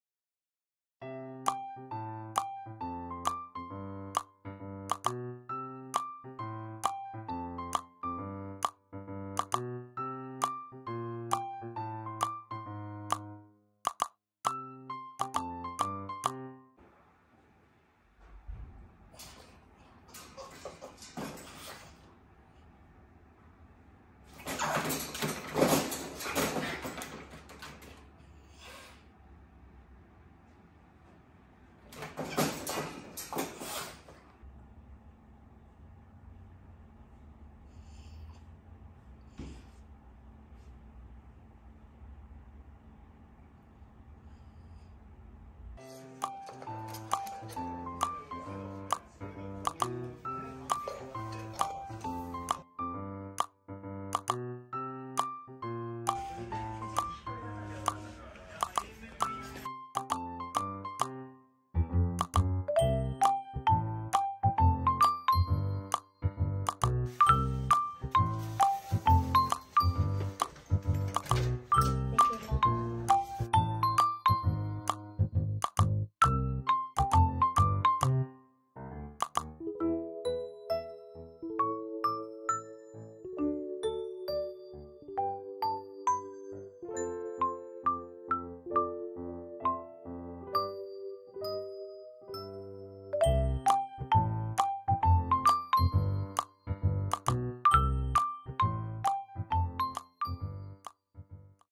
ピンポンが苦手なフレブル麦子
ピンポンが大の苦手、クイズ番組のピンポンも苦手。
鳴る度に興奮してしまいます。掃除機など機械音が全般苦手かも。
少しずつ慣れていって欲しいです。

動画ご視聴いただきありがとうございます！